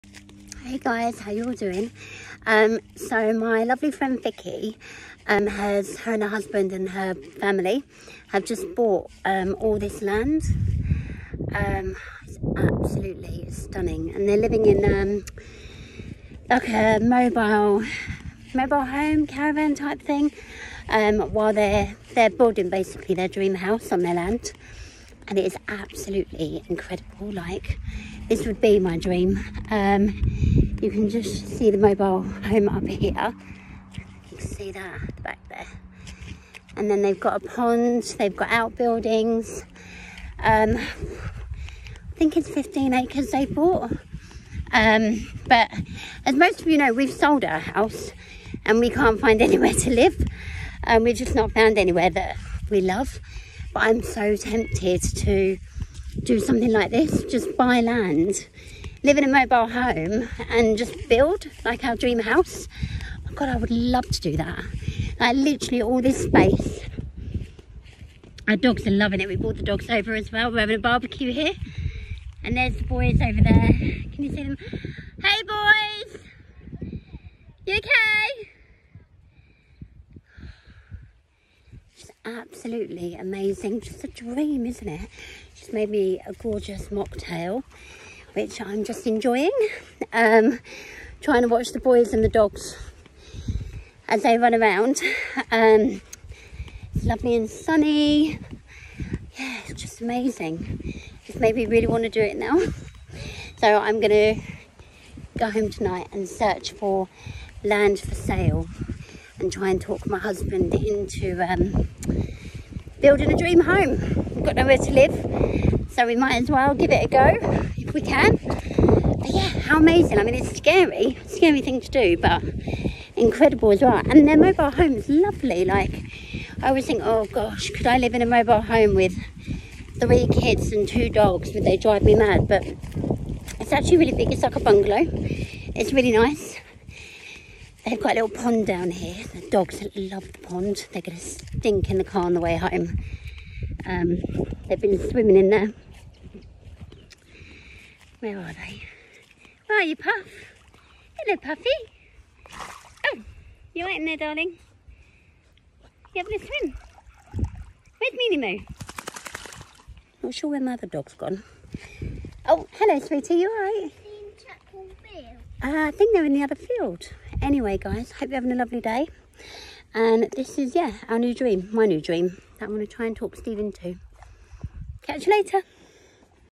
My Dream House 🏠
Lovely afternoon with our lovely friends @vpack88 ☀️

The boys and dogs had a whale of a time 🚜 thank you so much for having us!

How’s your day been guys?

𝗗𝗶𝗲𝘁 | 𝗠𝘂𝗺 | 𝗥𝗲𝗽𝗲𝗮𝘁